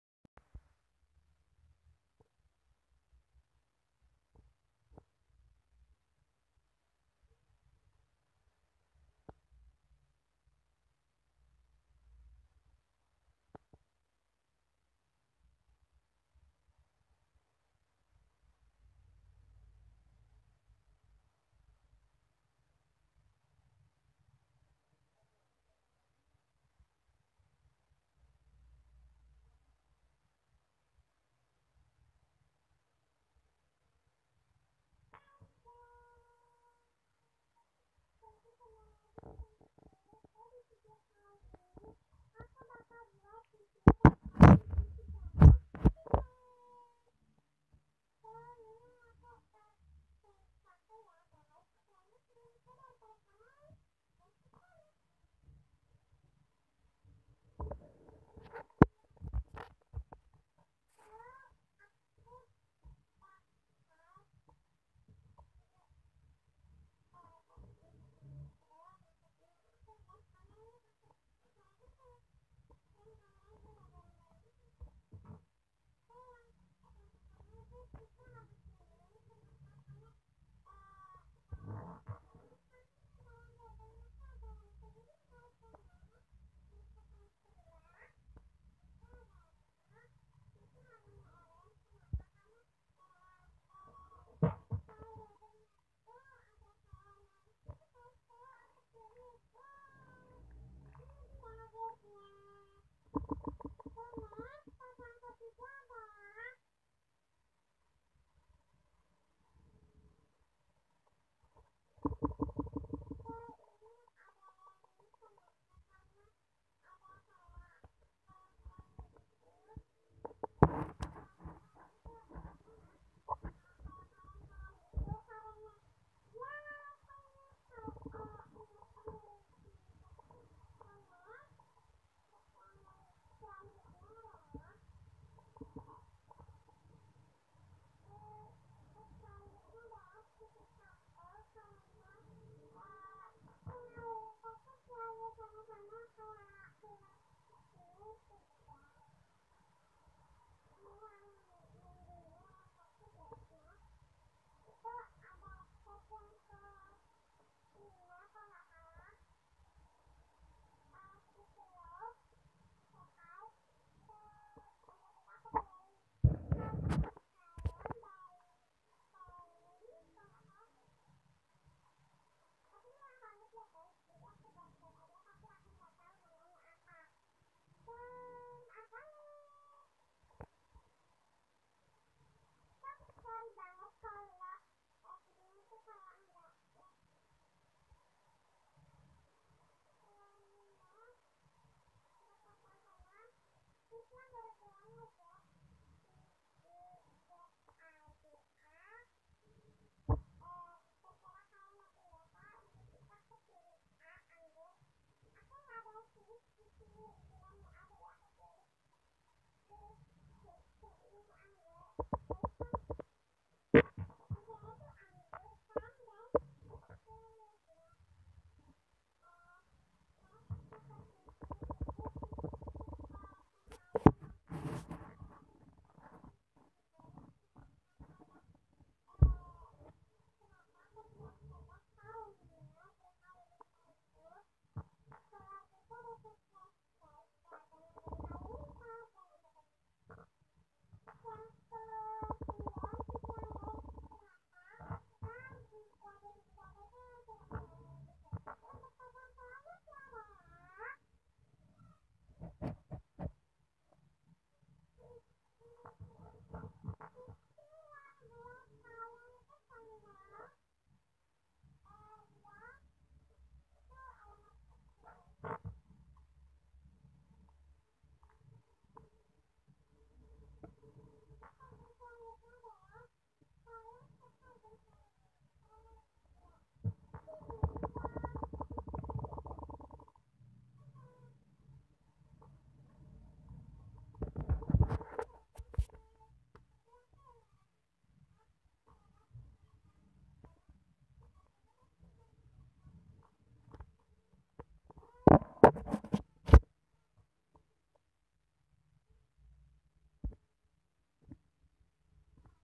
Zig zaga squihshy tag cr:by scr😋
Zig zaga squishy tag :
.squishy yg warnannya kuning
.squishy yg warnanya orange
.squishy keren
.squishy copel/kembar
.squishy love/love
.squihy yg huruf duluanya z,i,g,z,a,g,a
.squishy collection kalian yg ke 11
.squishy favorite kalian
.squishy yg rare sekarang
.squishy yg buat kalian senang
.kalian suka geng halilintar gak?

Cr:scr jr😆